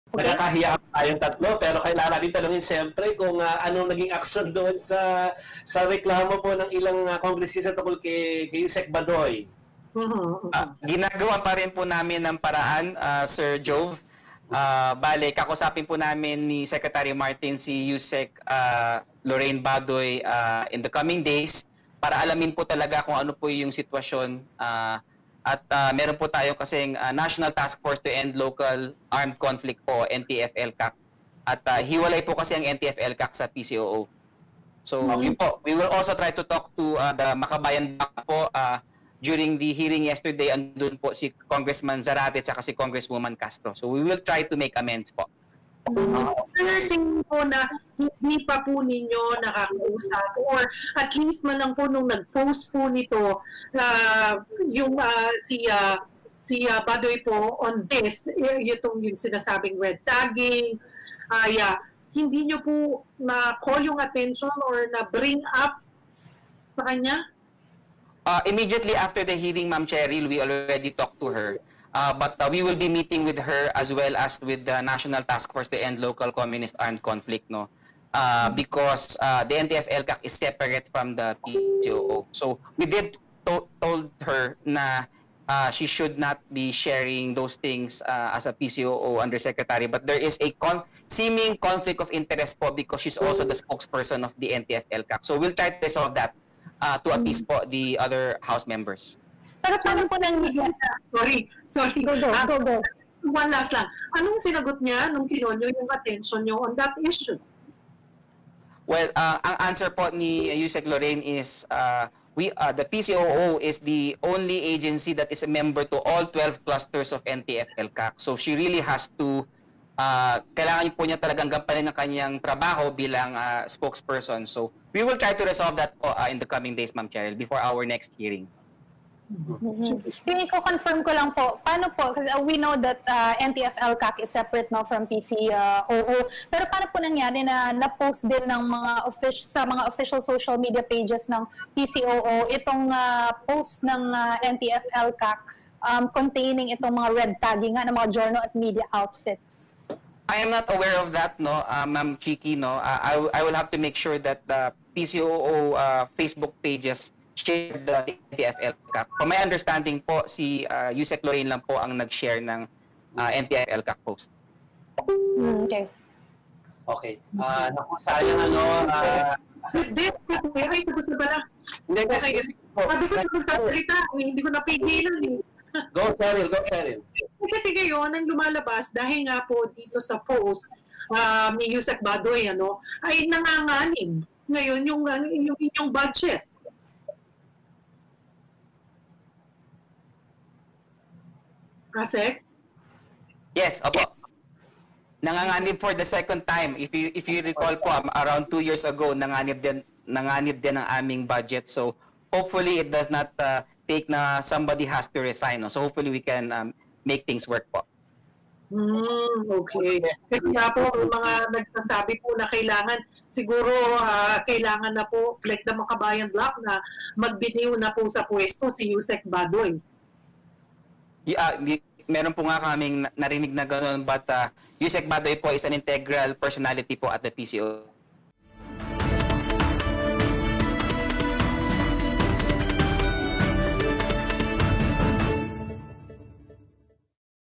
PCOO: 'Red-tagging' posts ni Usec. Badoy, sisikaping maresolba
Sinabi ni PCOO Asec. Kris Ablan na kailangan klaruhin ang posisyon ni Usec. Lorraine Badoy sa ahensya at ang pagiging miyembro nito ng NTF-ELCAC dahil sa umano'y "seeming conflict of interest."

Ito ay matapos manganib ang 2021 budget ng ahensya sa Kamara gawa ng "red-tagging" posts ni Badoy sa mga miyembro ng Makabayan bloc.